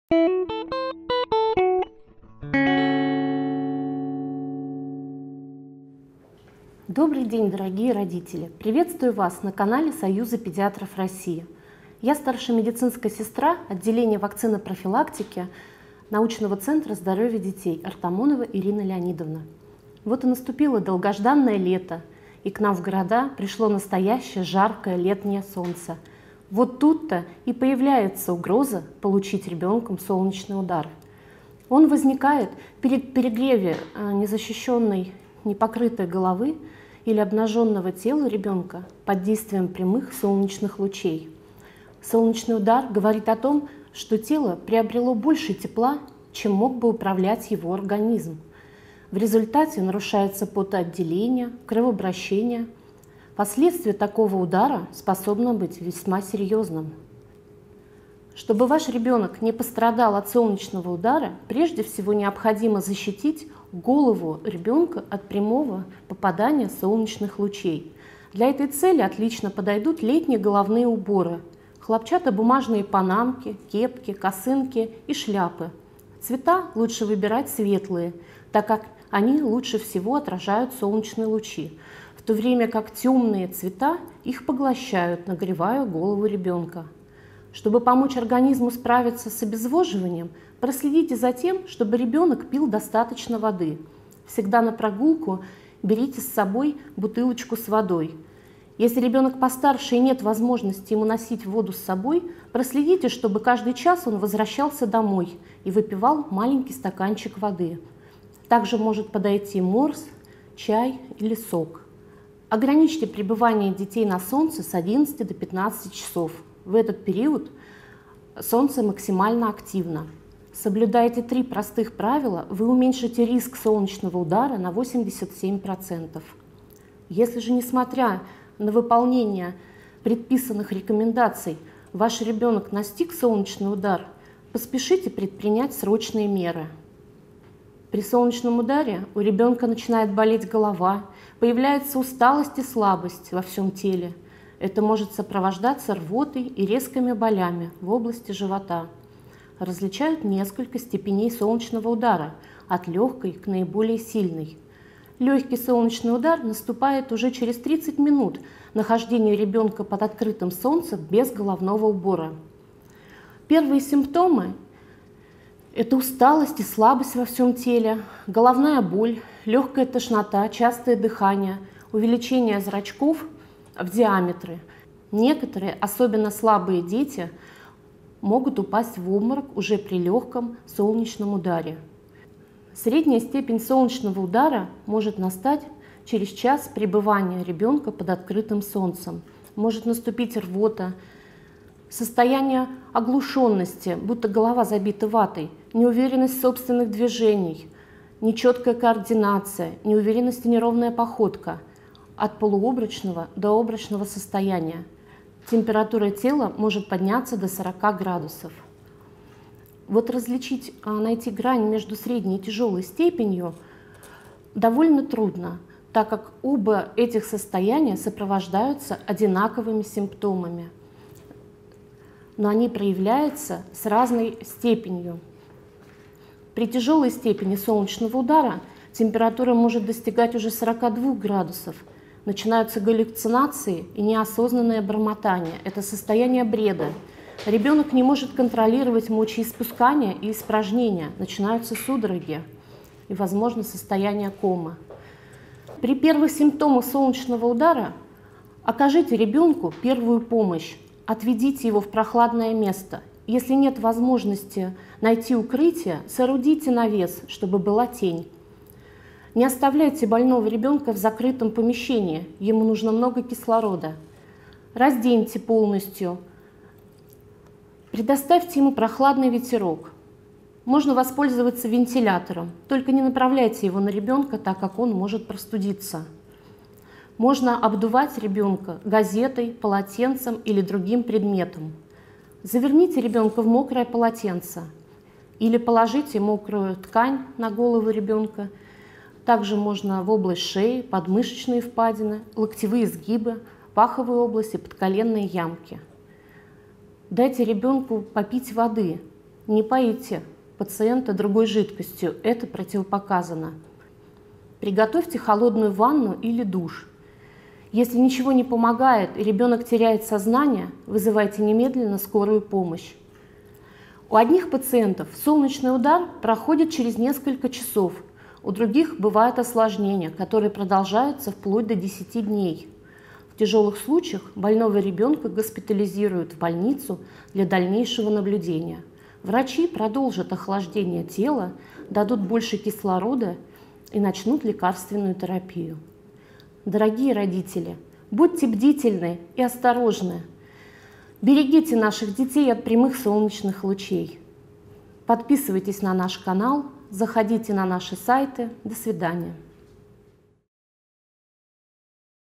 Солнечный удар - что нужно знать? Советы родителям - Союз педиатров России.
Союз педиатров России.

Солнечный удар - что нужно знать? Советы родителям.